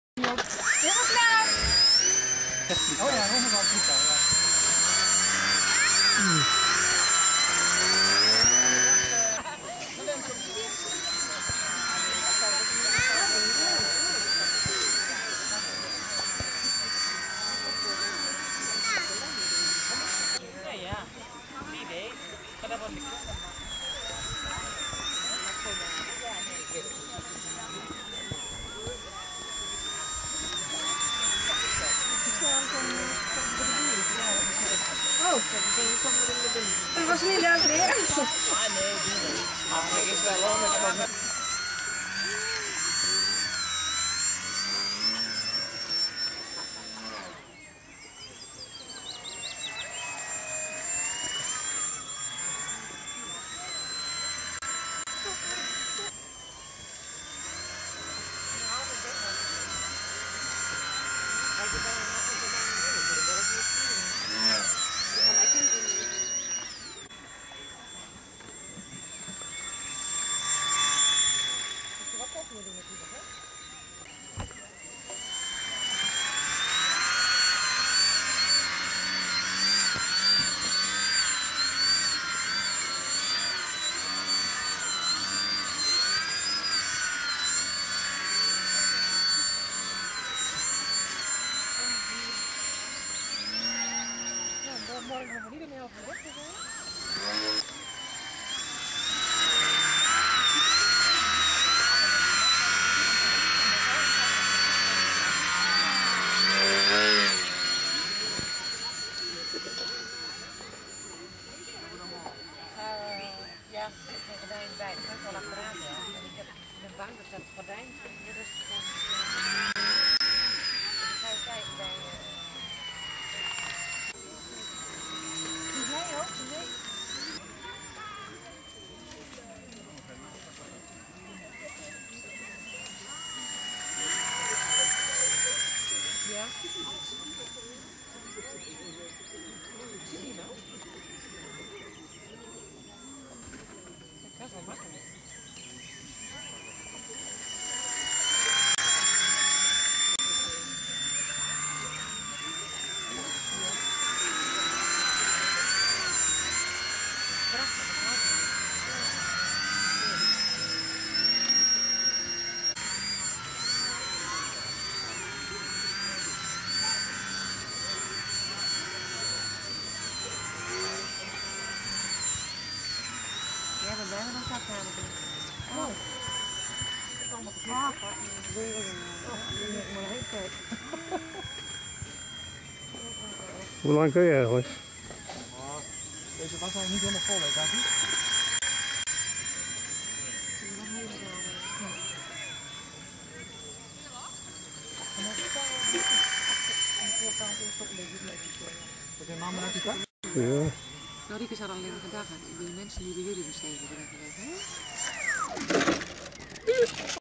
Kuierpadtien modelvliegen 27-5-2012
Modelvliegen op de camping Kuierpadtien te Wezuperbrug